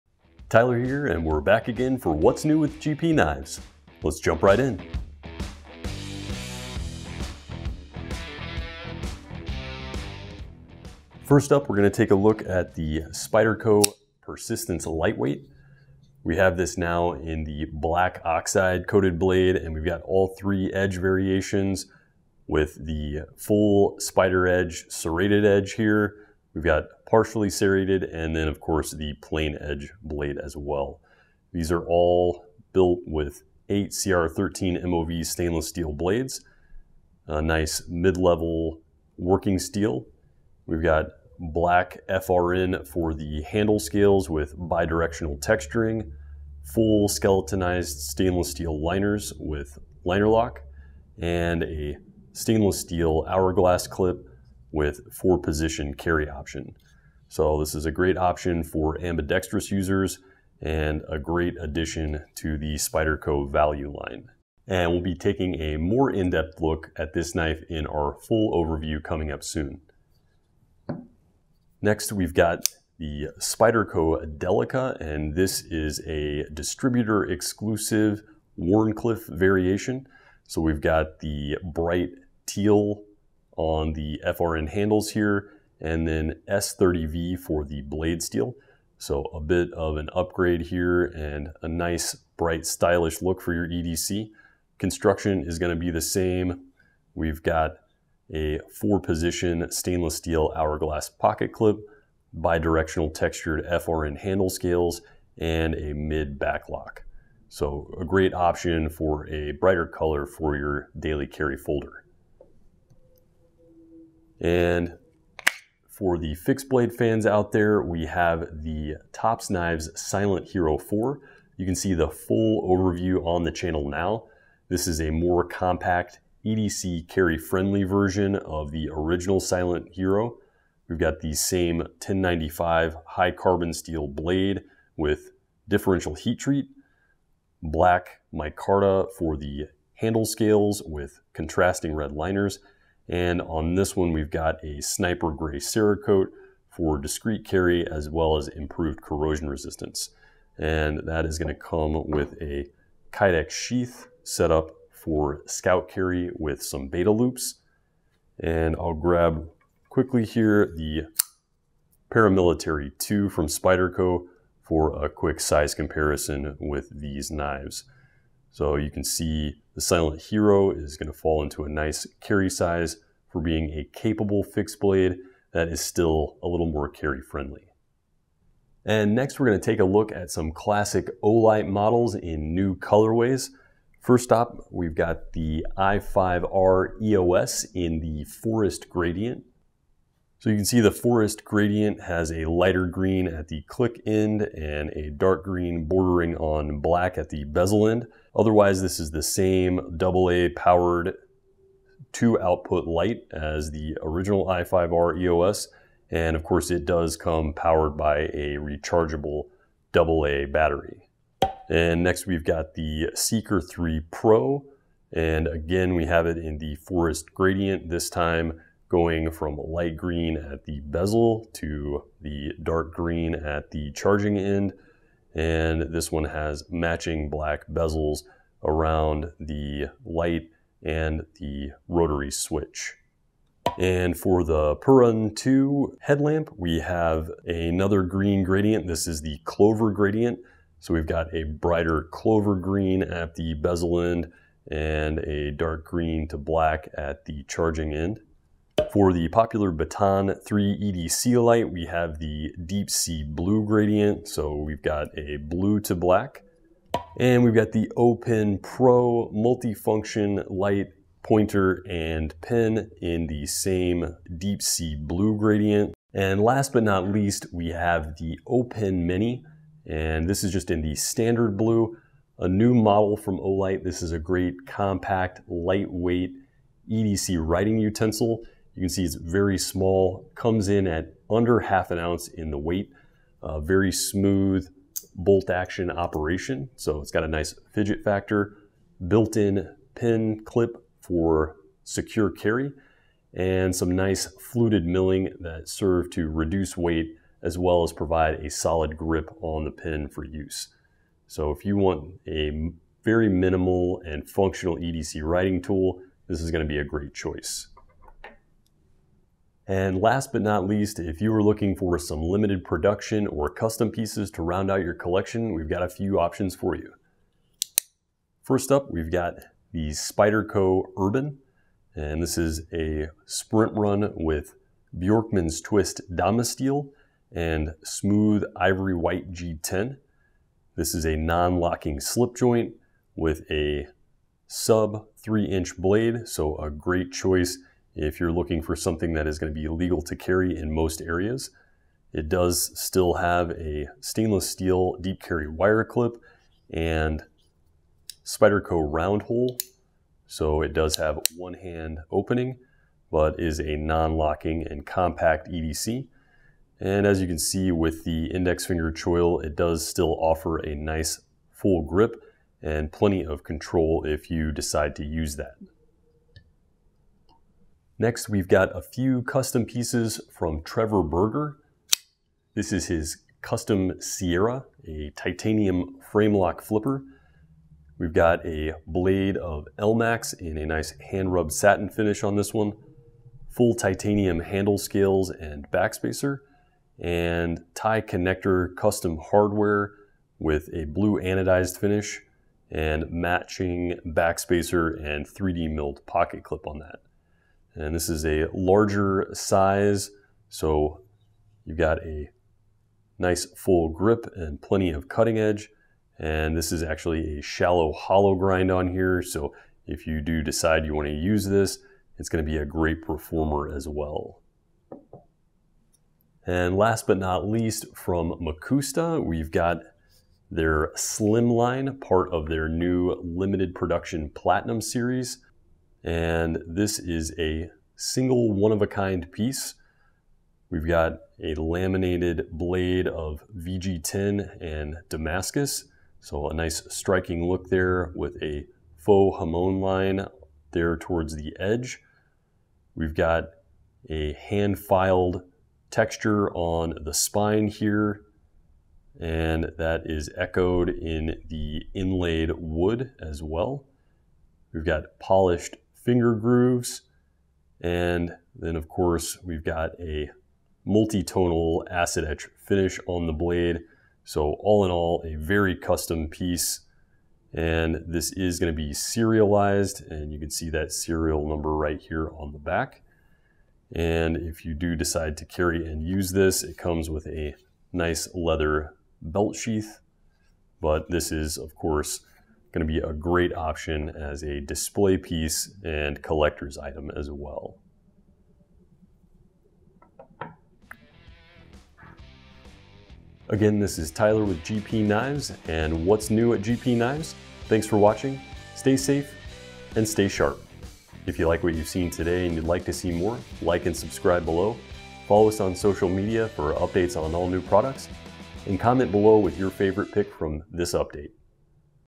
New at GPKNIVES | April 8th, 2022 | Spyderco, TOPS, Olight, and More!
Wanna stay up to date on the latest and greatest? Check out the newest items hitting the site!

Timestamps:
00:00  Intro
00:10  Spyderco Persistence Lightweight
01:11  S30V Teal Spyderco Delica
01:54  TOPS Silent Hero 4
02:54  Olight i5R EOS Forest
03:31  Olight Seeker 3 Pro Forest
03:53  Olight Perun 2 Clover
04:08  Olight Baton 3 Deep Sea Blue
04:17  Olight OPen Pro Deep Sea Blue
04:26  Olight O'Pen Mini Blue
05:18  Spyderco Urban Sprint
06:07  Trevor Burger Custom Sierra
06:56  Mcusta Platinum Label Slim Line
08:13  Sign Off